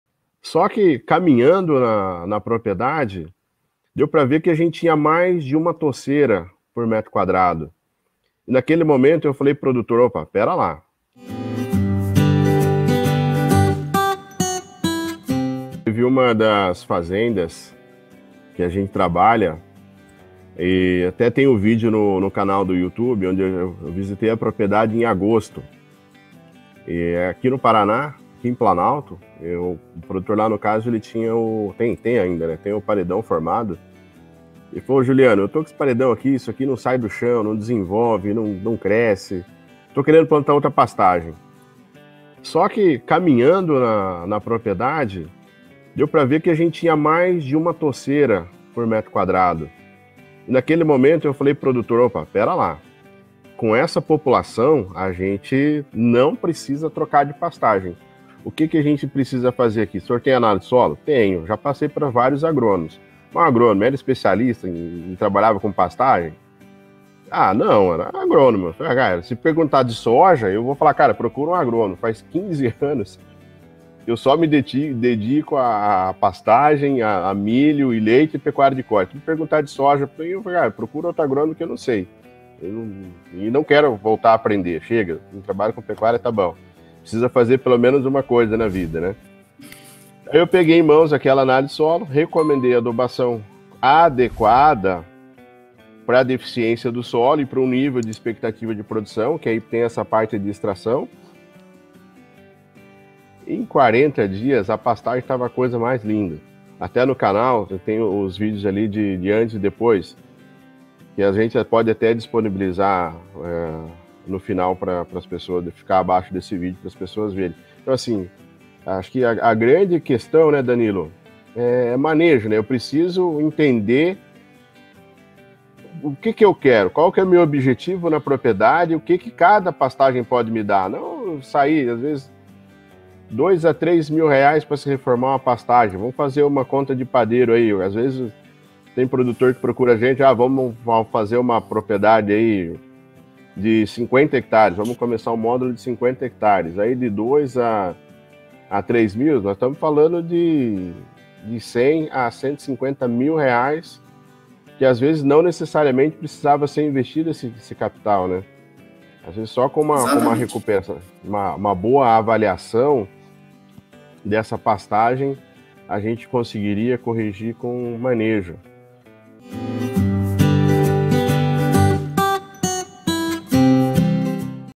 Reformar ou melhorar o manejo de pastagem qual o melhor - Doutor Pastagem
Uma dúvida frequente que recebo de vários pecuaristas é:

Reformar ou melhorar o manejo de pastagem? Qual o melhor?

Essa dúvida respondo juntamente com meu amigo, Agrônomo Danilo Fonseca.

Um bom vídeo a todos.

Juliano Alarcon - Doutor Pastagem